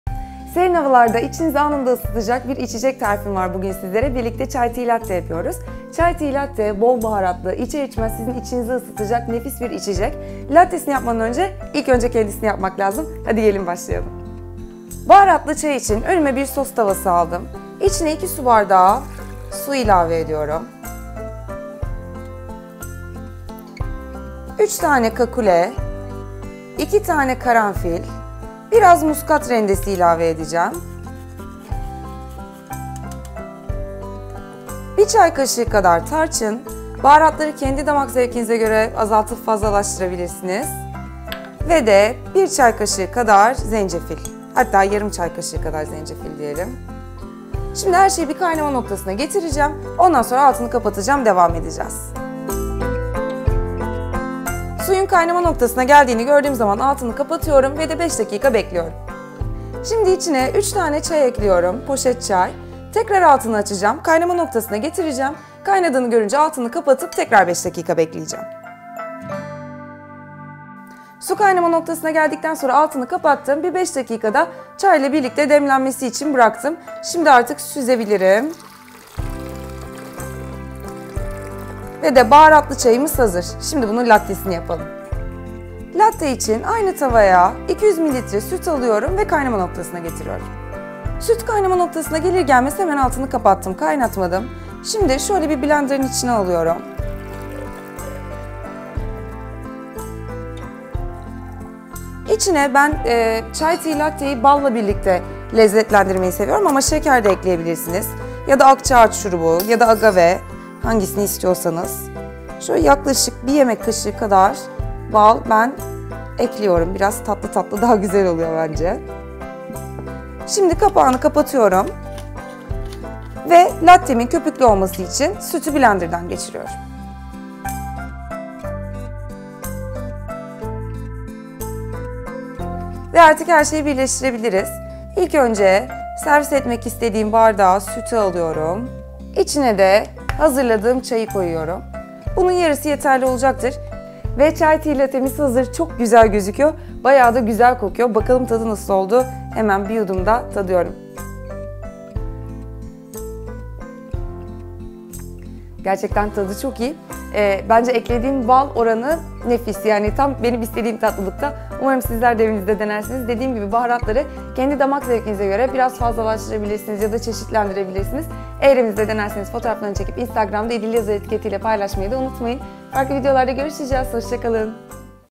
Chai Tea Latte Nasıl Yapılır? - İdil Yazar - Yemek Tarifleri
Chai Tea Latte Tarifi.

Serin havalarda içinizi ısıtacak Chai Tea Latte tarifi.

Chai Tea Latte için Malzemeler:

Malzemeler:
2 bardak Su
3 adet Kakule
2 adet Karanfil
Muskat Rendesi
1 çay kaşığı Tarçın
1/2 çay kaşığı Zencefil
3 adet poşet Çay
200 ml Süt
1 yemek kaşığı Bal